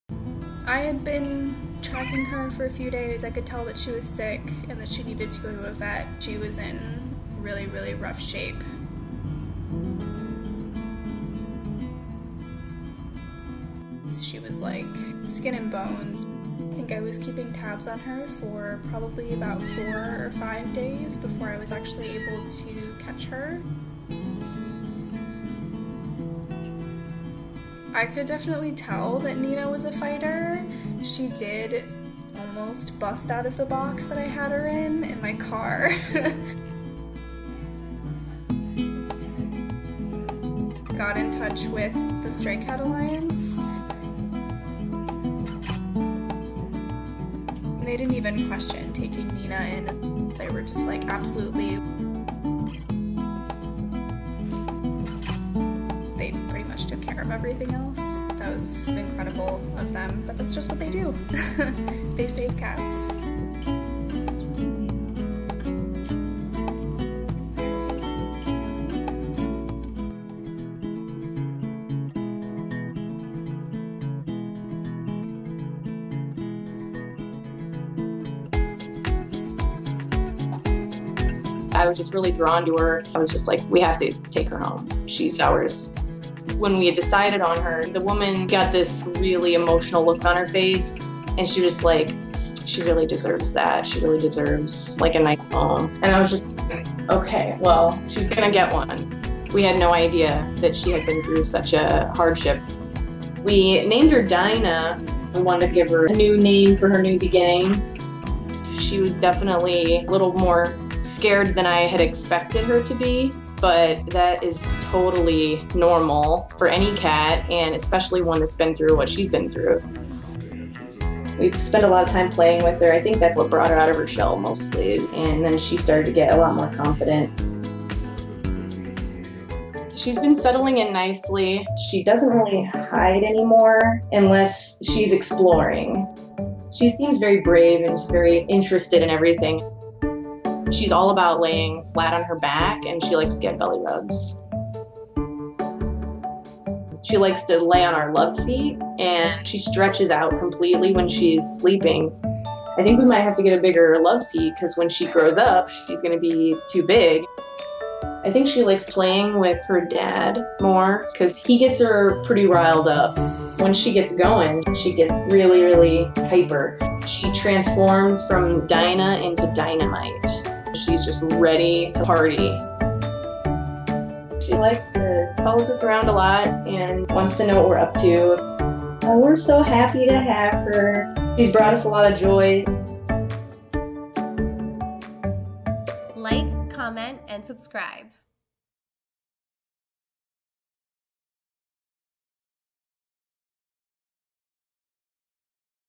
Stray Cat Completely Transforms In Her New Home | The Dodo
Molly Sotak found a kitten on the ground outside her apartment building and took her to the Stray Cat Alliance. There, the kitten was taken care of until she was adopted by a loving family and named Dinah.

You can keep up with Dinah and all of her adventures on Instagram You can help Stray Cat Alliance care for more cats like Dinah by donating You can also check them out on Instagram and Facebook